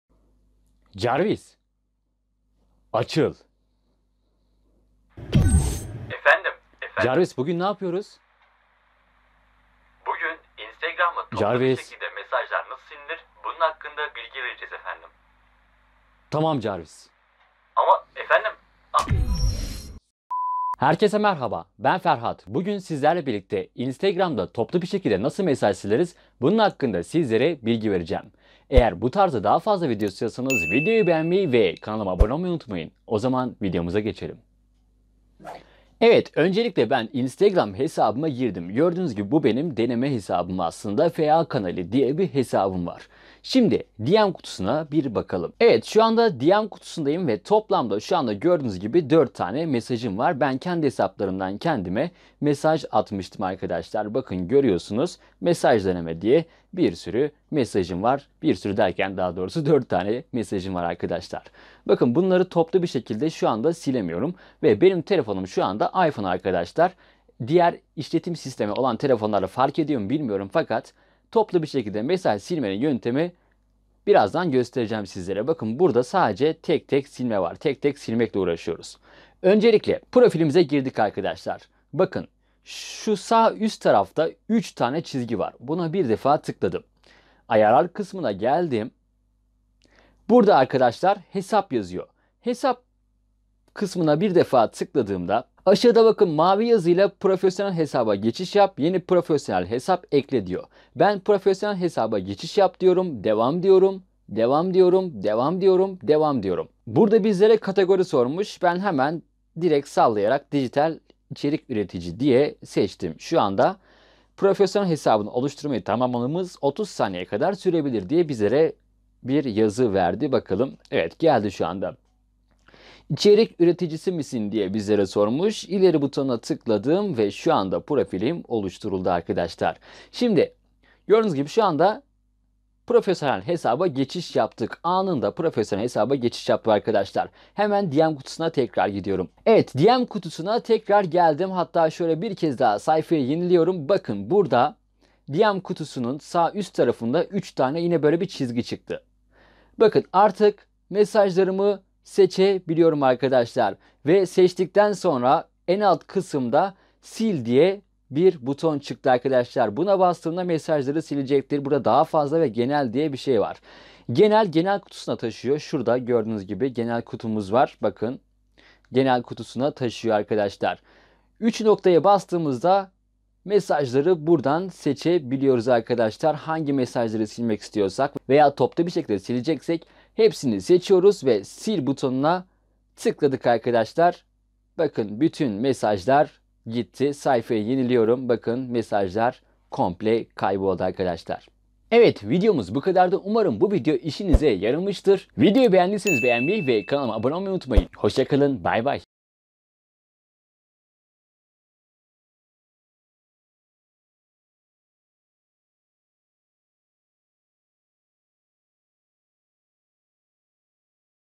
İNSTAGRAM TOPLU MESAJ SİLME | instagram toplu mesaj nasıl silinir
İNSTAGRAM TOPLU MESAJ SİLME | instagram toplu mesaj nasıl silinir adlı videomla sizlerleyim.

Merhaba, ben Ferhat Aymaz. Kanalımda eğitim ve tavsiye videoları hazırlıyorum.